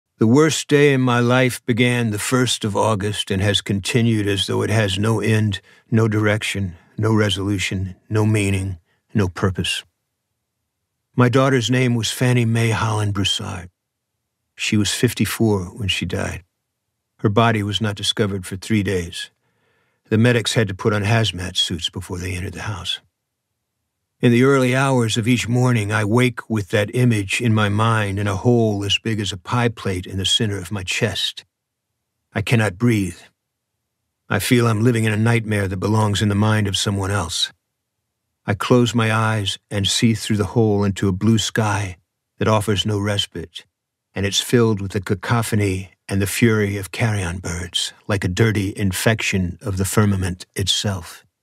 Listen to EVERY CLOAK ROLLED IN BLOOD
Listen to an excerpt from EVERY CLOAK ROLLED IN BLOOD by James Lee Burke and read by Will Patton.

In his most autobiographical novel to date, James Lee Burke continues the epic Holland family saga with a writer grieving the death of his daughter while battling earthly and supernatural outlaws.